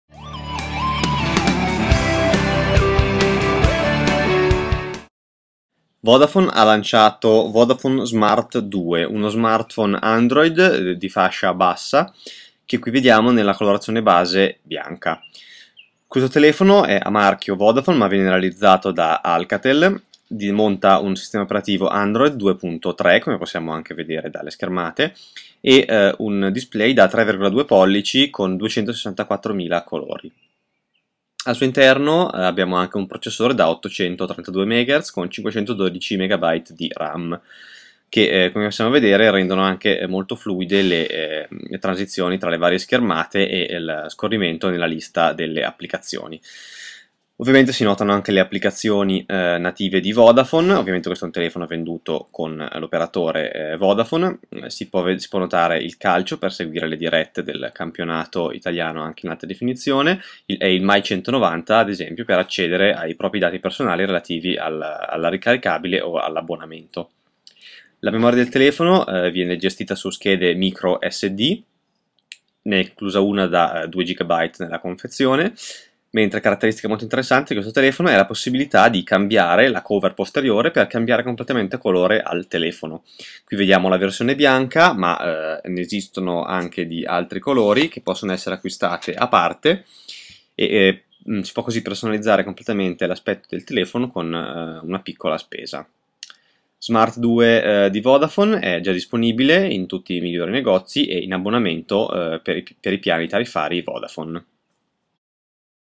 Vodafone Smart 2
Smartphone Android 2.3 con processore da 832 MHz e 512 MB di RAM. Memoria espandibile tramite schede micro SD (una da 2 GB è inclusa nella confezione) e cover intercambiabili colorate.
Il telefono è dotato di connettività 3G HSDPA, Wi-Fi e GPS, mentra la fotocamera è da 3.2 Megapixel.